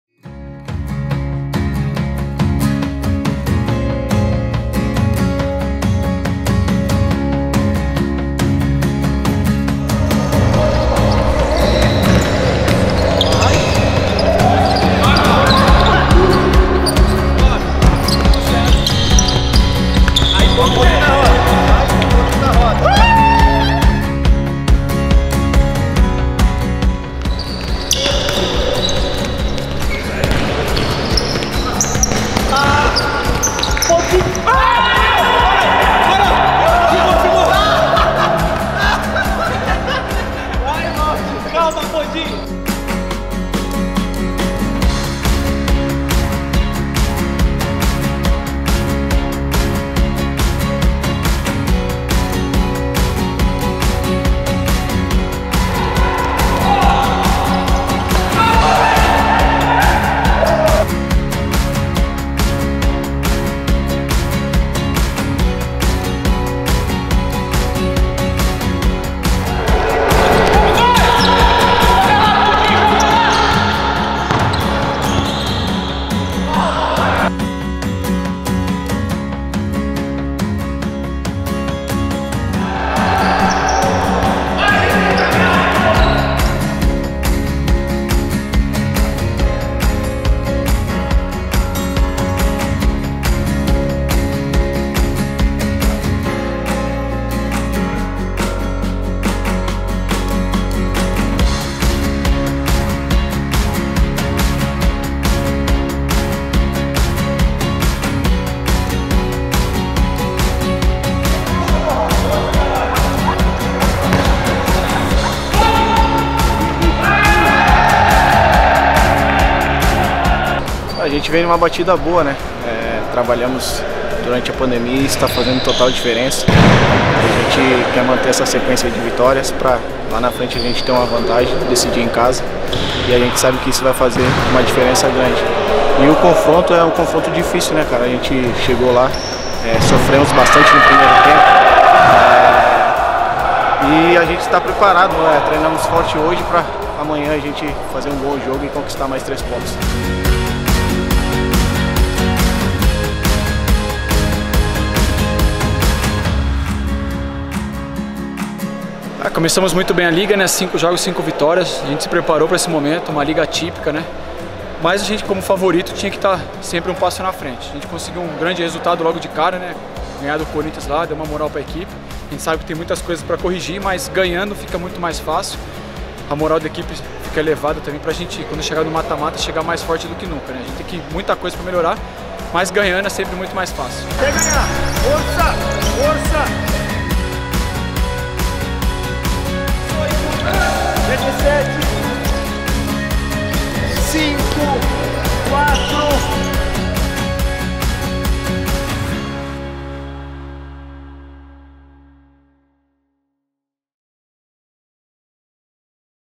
Pré Jogo - Magnus Futsal x Brasília - Liga Nacional 2020
Logo após um jogo difícil fora de casa e mais de 12 horas de viagem, nosso time fez o chamado "peladão" antes de mais um confronto contra o Brasília, dessa vez em Votorantim (SP). A partida acontece neste sábado (19/09), a partir das 11h, com transmissão ao vivo da TV NSports e Rádio Magnus

🏆 Liga Nacional 2020
⚽️ Magnus Futsal x Brasília
🏟 Gin. João Carlos de Camargo - Votorantim/SP
📅 19/09
⏰ 11:00
⛔️ Portões fechados

📺 Pay-per-view TV NSports ou app disponível para iOS e Android.

ENTREVISTA
O ala Pett e o capitão Rodrigo falaram sobre a partida.